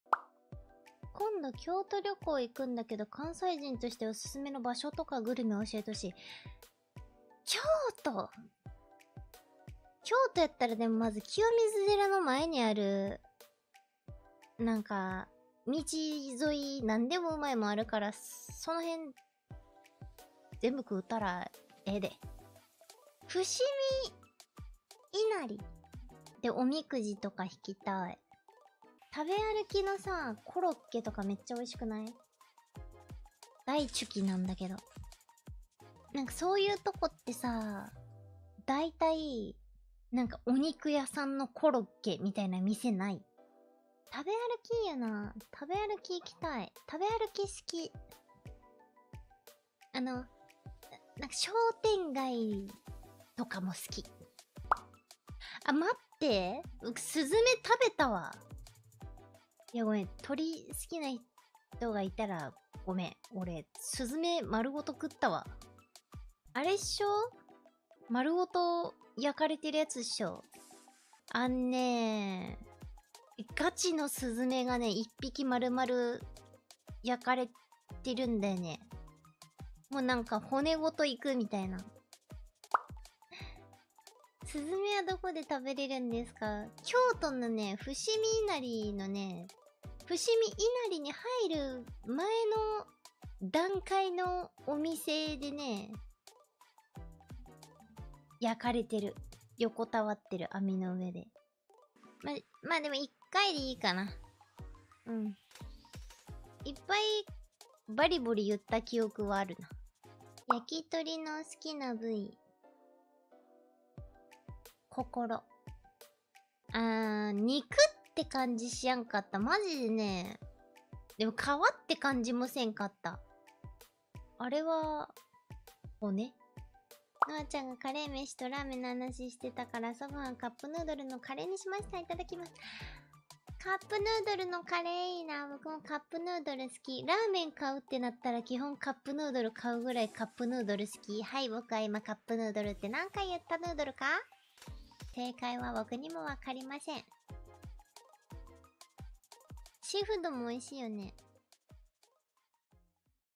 ○○○を食べたことがある胡桃のあに驚愕するリスナーたち！ｗ【ぶいすぽ！/胡桃のあ】
元動画
おはようといってらっしゃい